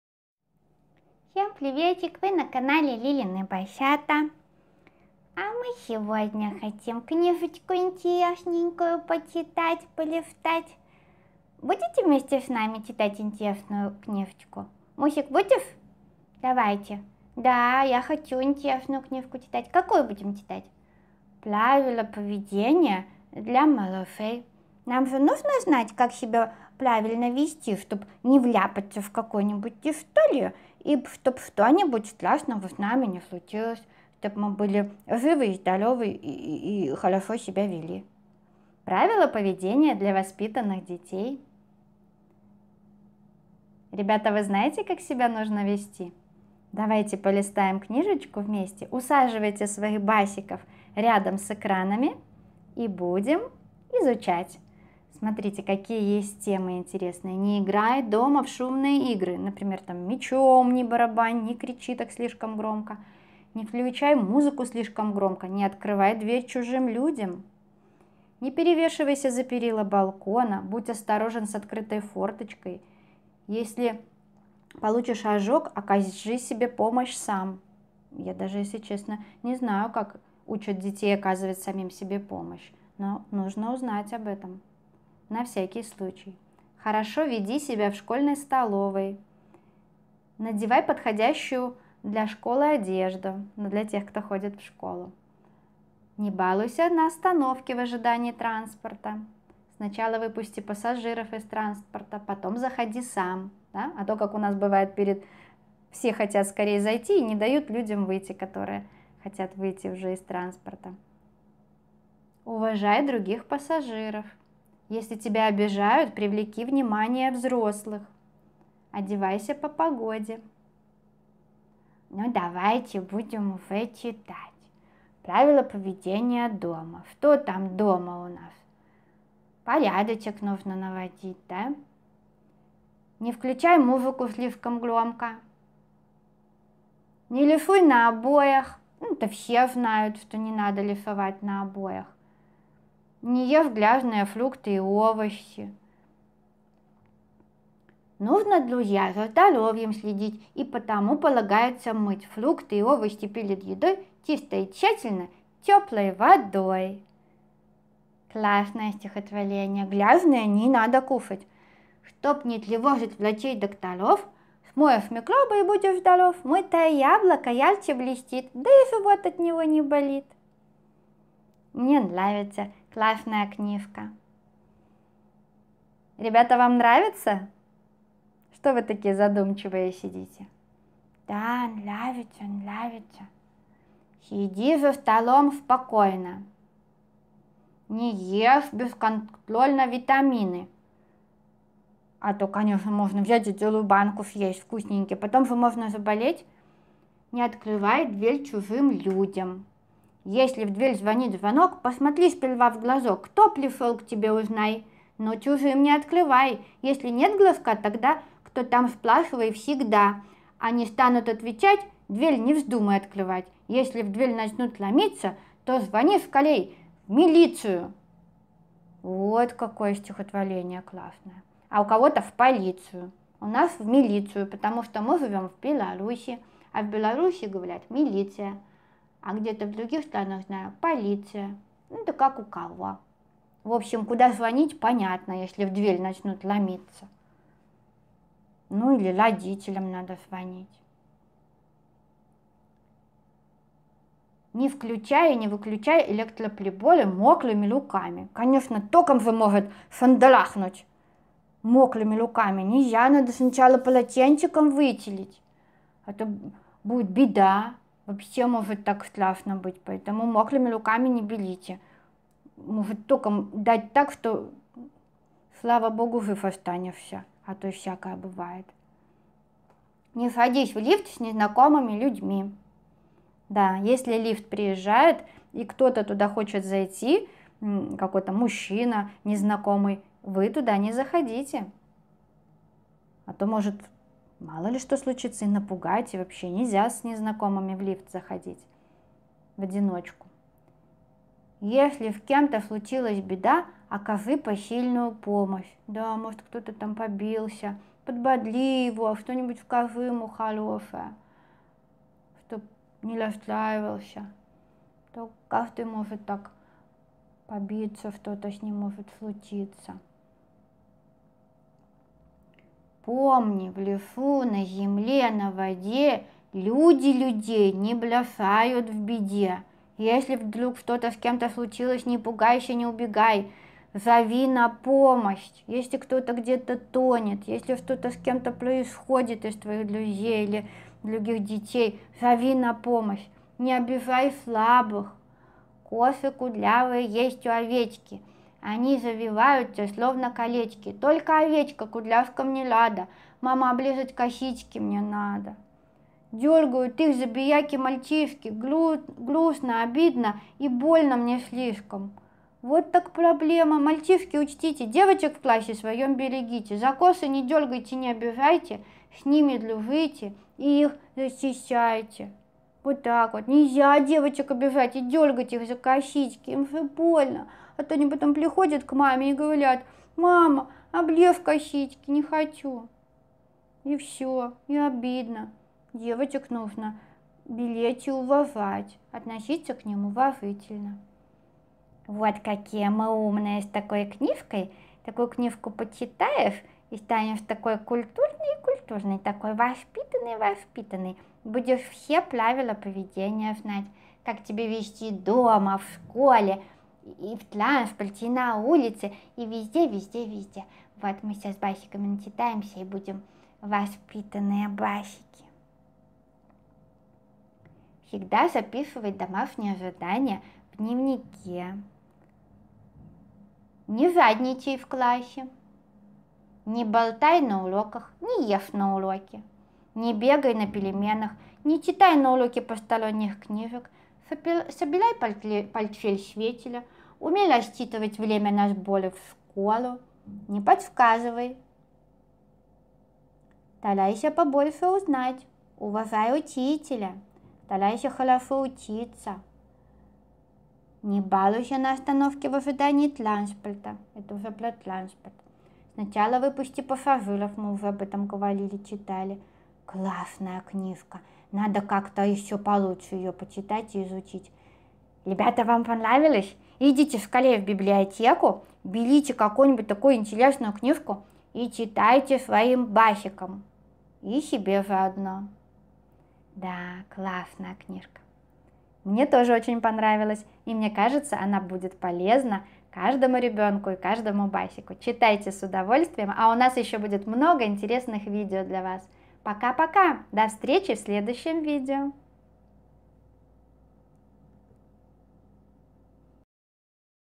Басики читают книжку "Правила поведения"
Басики любят читать книги и любят когда им читают. Сегодня замечательный день, у басиков замечательное настроение. Мы читаем интересную книгу как правильно себя вести. Это очень важно знать всем басикам и всем детям. Ребята, усаживайте своих басиков, включайте им это видео, пусть учатся правильному поведению. Тогда они будут культурными, воспитанными и не попадут в просак.